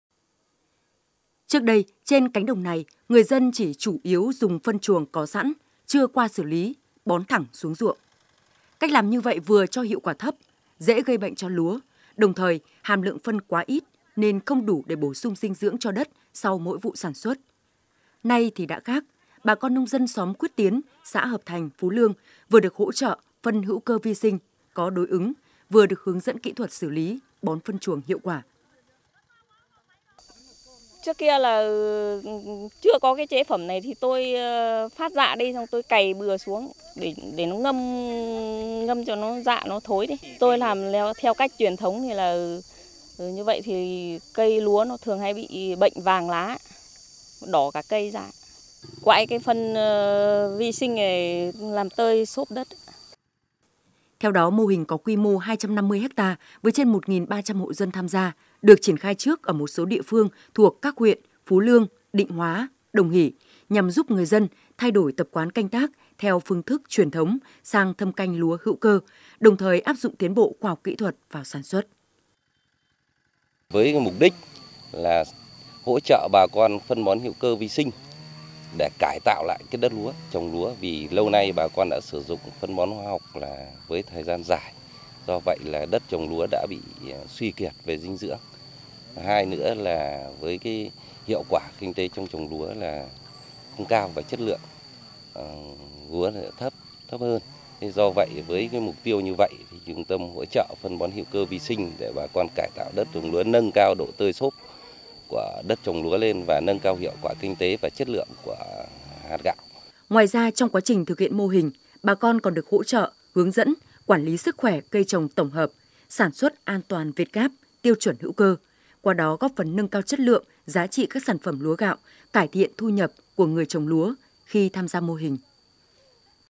Hỗ trợ nông dân sản xuất hữu cơ
Trước đây, việc lạm dụng phân bón hóa học trong sản xuất nông nghiệp đã làm đất bạc màu, giá trị nông sản suy giảm và tác động không tốt tới sức khỏe của người sản xuất, tiêu dùng. Để hạn chế tình trạng này và thay đổi thói quen canh tác cho bà con nông dân, Trung tâm Khuyến nông tỉnh Thái Nguyên đã triển khai mô hình sản xuất lúa ứng dụng tiến bộ khoa học kỹ thuật gắn với tăng cường sử dụng phân bón hữu cơ.